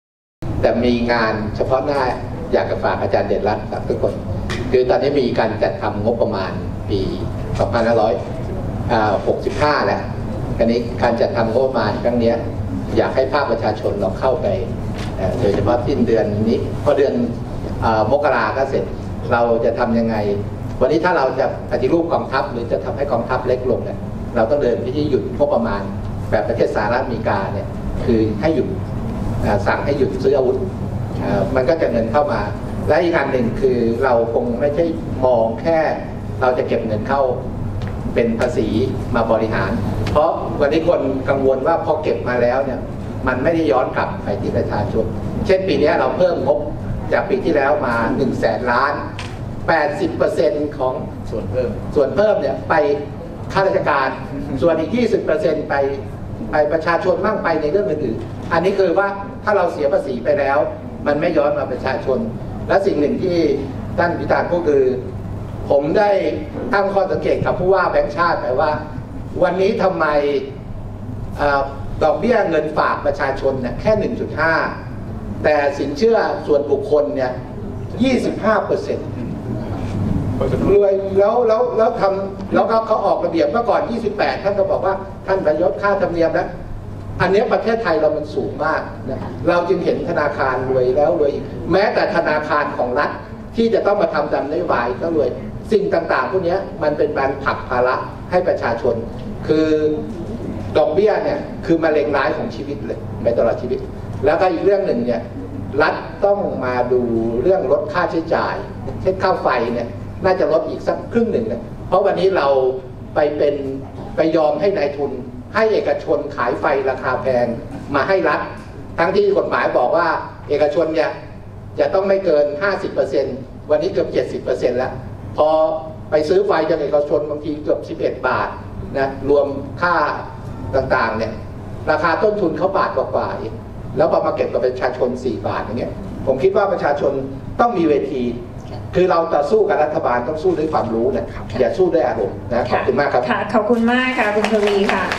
"ทวี" แนะรัฐลดความเหลื่อมล้ำ สร้างสิทธิเท่าเทียม-มุ่งสู่รัฐสวัสดิการ
เครือข่ายประชาชนเพื่อรัฐสวัสดิการ จัดงาน “มหกรรมรัฐธรรมนูญใหม่สู่รัฐสวัสดิการ: ประชาธิปไตย รัฐธรรมนูญ รัฐสวัสดิการ” ภายในงานมีกิจกรรมต่างๆ รวมทั้งการเสวนา “ความเป็นไปได้ของการจัดสรรงบประมาณในประเทศเพื่อจัดรัฐสวัสดิการ” นำโดย พันตำรวจเอก ทวี สอดส่อง สมาชิกสภาผู้แทนราษฎร (ส.ส.) และเลขาธิการพรรคประชาชาติ นายพิธา ลิ้มเจริญรัตน์ หัวหน้าพรรคก้าวไกล นายเดชรัต สุขกำเนิด นักวิชาการอิสระ และนายนิมิตร์ เทียนอุดม เครือข่ายประชาชนเพื่อรัฐสวัสดิการ ดำเนินรายการโดย นางสาวหทัยรัตน์ พหลทัพ บรรณาธิการเดอะอีสานเรคคอร์ด ทั้งนี้มีประชาชนเข้าร่วมงานเสวนาเป็นจำนวนมาก ณ หอศิลปวัฒนธรรมแห่งกรุงเทพมหานคร

พันตำรวจเอก ทวี สอดส่อง สมาชิกสภาผู้แทนราษฎร (ส.ส.) และเลขาธิการพรรคประชาชาติ ระบุว่า “วันนี้คิดว่ามีความเป็นไปได้ของรัฐสวัสดิการ แต่ก่อนที่จะถึงเรื่องความเป็นไปได้นั้น ก็มีความยาก ความท้าทาย เพราะการปกครองในระบบประชาธิปไตยอันมีพระมหากษัตริย์ทรงเป็นประมุขของไทย เพราะเราถือรัฐธรรมนูญนิยม ซึ่งคือรัฐธรรมนูญต้องเป็นลายลักษณ์อักษร ดังนั้นต้องยอมรับว่าเมื่อเป็นรัฐธรรมนูญนิยมจะมีการแย่งชิงผลประโยชน์ ซึ่งประเทศไทยนั้นมี 4 รัฐมาโดยตลอด คือ 1.รัฐประหาร 2.รัฐธรรมนูญ 3.รัฐสภา 4.รัฐบาล แต่ในรัฐธรรมนูญฉบับปัจจุบันยังมีรัฐอิสระที่คอยจัดการตัวแทนของประชาชนที่มาจากการเลือกตั้ง ซึ่งเป็นองค์กรอิสระที่มาตามรัฐธรรมนูญที่ไม่ได้มาจากประชาชนแต่มีอำนาจมาก”

พ.ต.อ.ทวี กล่าวในช่วงท้ายว่า "มีงานเฉพาะด้าน อยากจะฝากอาจารย์เดชรัตน์กับทุกคน คือตอนนี้มีการจัดตั้งงบประมาณปี 2565 การจัดทำงบประมาณครั้งนี้อยากให้ภาคประชาชนเข้าไป โดยเฉพาะสิ้นเดือนนี้พอเดือนมกราคมก็เสร็จ เราจะทำยังไงวันนี้ถ้าเราเอาแต่ปฏิรูปกองทัพหรือจะทำให้กองทัพเล็กลง เราต้องเดินไปหยุดงบประมาณแบบประเทศสหรัฐอเมริกา และอีกอันคือเราคงไม่ใช่มองแค่เราจะเก็บเงินเข้าเป็นภาษีมาบริหาร เพราะวันนี้คนกังวลว่าพอเก็บมาแล้วมันไม่ได้ย้อนกลับไปที่ประชาชน เช่น ปีนี้เราเพิ่มงบจากปีที่แล้วมาหนึ่งแสนล้านบาท 80% เพิ่มไปยังข้าราชการ ส่วนอีก 20% ไปยังประชาชน ถ้าเราเสียภาษีไปแล้วมันไม่ย้อนกลับมาหาประชาชน และสิ่งหนึ่งที่ท่านพิธาพูดคือ ผมได้ตั้งข้อสังเกตกับผู้ว่าแบงก์ชาติไปว่า ครั้งนี้ทำไมดอกเบี้ยเงินฝากประชาชนแค่ 1.5% แต่สินเชื่อส่วนบุคคล 25% แล้วเขาออกระเบียบว่าก่อน 28% ท่านไปยกค่าธรรมเนียม อันนี้ประเทศไทยเรามันสูงมาก เราจึงเห็นธนาคารรวยแล้วรวยอีก แม้แต่ธนาคารของรัฐที่จะเข้ามาทำตามนโยบายก็รวย สิ่งต่างๆพวกนี้มันเป็นการเอาเปรียบประชาชน คือ ดอกเบี้ยเป็นมะเร็งร้ายของชีวิตไปตลอดชีวิต แล้วอีกอย่างหนึ่งรัฐต้องมาดูเรื่องลดค่าใช้จ่าย เช่นค่าไฟน่าจะลดอีกซักครึ่งหนึ่ง วันนี้เรายอมให้นายทุนยอมให้เอกชนขายไฟราคามาแพงมาให้รัฐ ทั้งที่กฎหมายบอกว่า เอกชนจะต้องไม่เกิน 50% วันนี้เกือบ 70% แล้ว ไปซื้อไฟกับเอกชนบางที 11 บาท รวมค่าต่างๆ ราคาต้นทุนเขา 1 บาทกว่าๆเอง แล้วมาเก็บกับประชาชน 4 บาท ผมคิดว่าประชาชนต้องมีเวที เราจะสู้กับรัฐบาลต้องสู้ด้วยความรู้ อย่าสู้ด้วยอารมณ์"